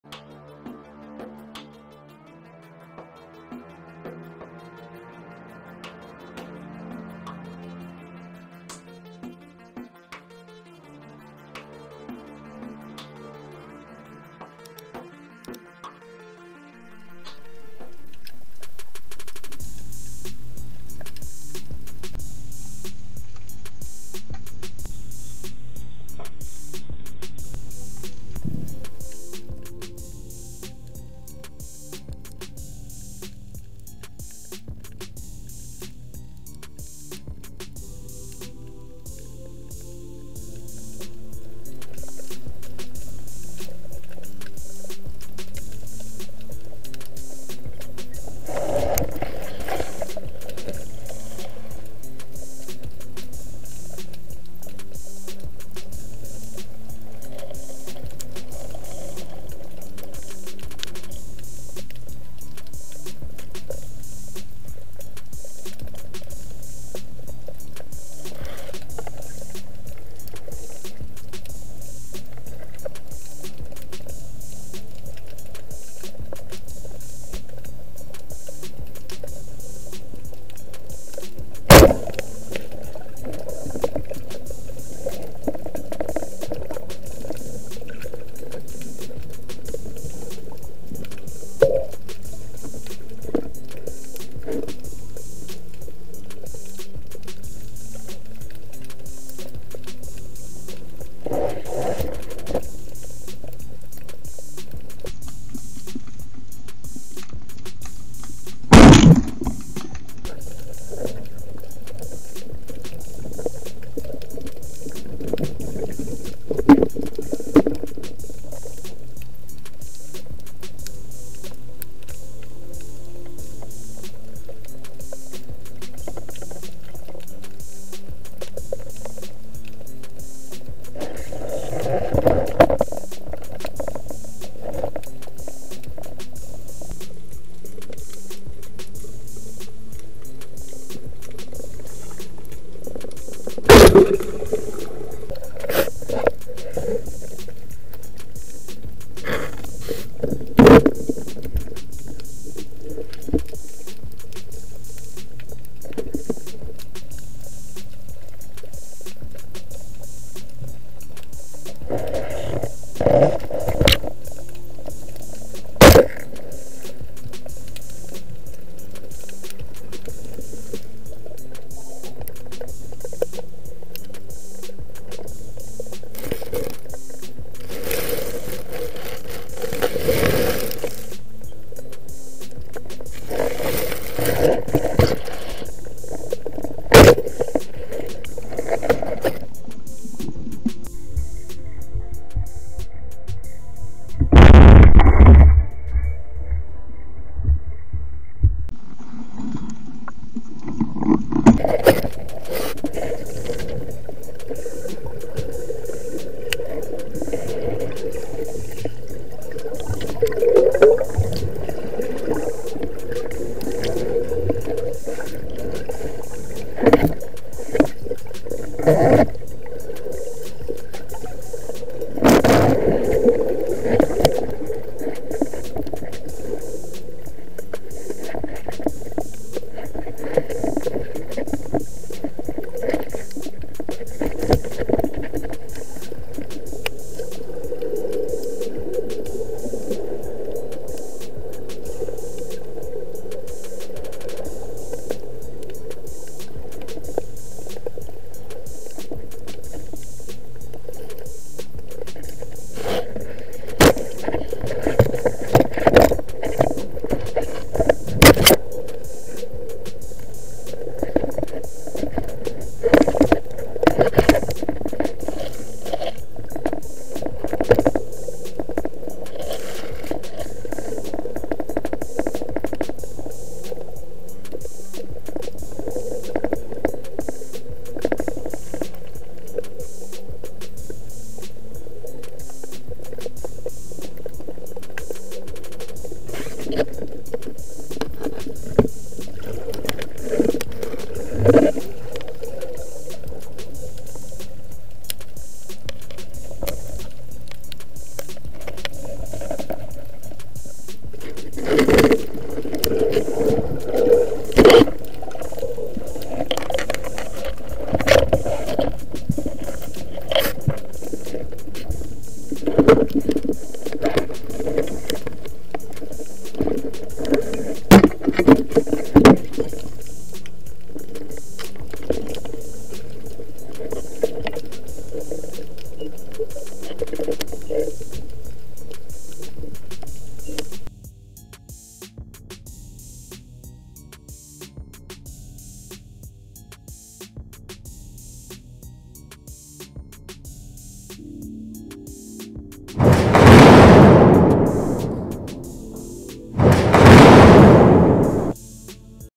Canavar Levrek Avı (Karadeniz Balık Avları)
Merhabalar, uzun süredir paylaşım yapamadım. Hem iş yoğunluğu hem de bazı teknik aksaklıklar nedeniyle ara vermiştim. Ancak tekrar dönüyorum inşallah. Güzel bir akşam suyu yaptım, kefaller çiftleşmeden yeni çıktığı için şuan kışa hazırlık amaçlı sürekli yemlenme yapıyorlar. Bu nedenle sarı kulak kefal avı yapmak için son zamanları iyi değerlendirmek istedim. Verimli bir av oldu, artık sudan çıkmaya yakın bir anda karşıma bir dev çıktı, 6 kiloya yakın gelen bu güzel levreği sizlere gösterebilme imkanını Allah' ım bana nasip ettiği için şükürler olsun. Şöyle bir sıkıntı oldu, levreğin o an yaşattığı aksiyonla zıpkın kasetini tutan pimin oynadığını göremedim, zıpkını tam kurarken pim yerinden fırladı, kaset tetik mekanizması bir tarafa şiş başka bir yana herşey dağıldı. Şükür hepsini buldum ancak şiş kırılmıştı. Kaseti de sağolsun Necmi AKDOĞAN abimiz yerine taktı hata yeniden pim bile yaptı benim için. Ne kadar teşekkür etsem kendisini azdır. İyi seyirler diliyorum.